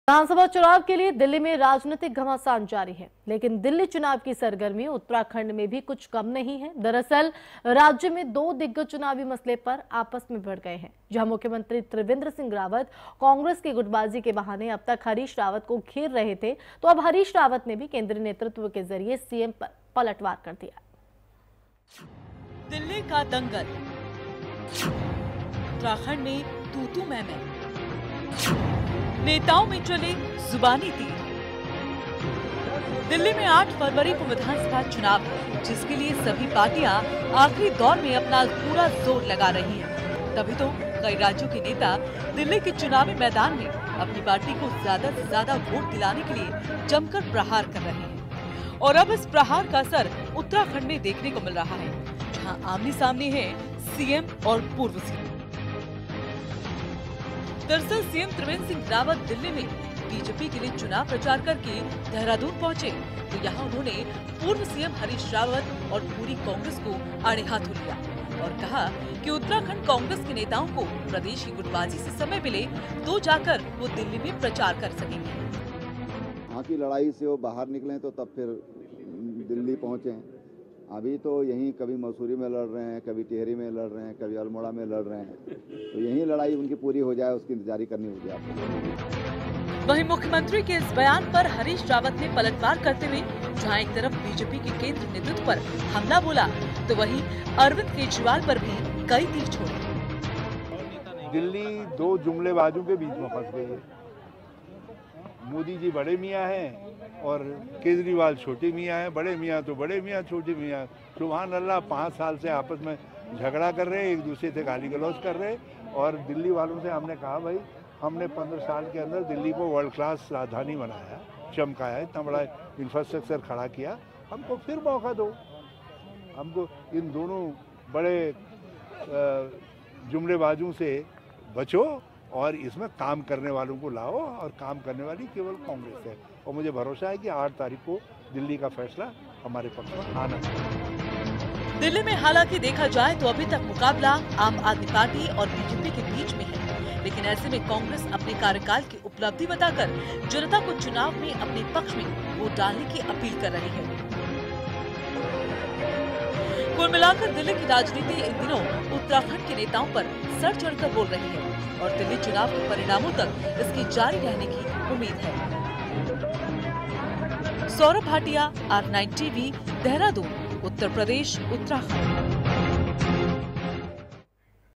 उत्तराखंड में दिल्ली की चुनावी गर्मी, खूब चले जुबानी तीर R9TV
विधानसभा चुनाव के लिए दिल्ली में राजनीतिक घमासान जारी है…लेकिन दिल्ली चुनाव की सरगर्मी उत्तराखंड में भी कुछ कम नही...दरअसल राज्य में दो दिग्गज चुनावी मसले पर आपस में भिड़ गए.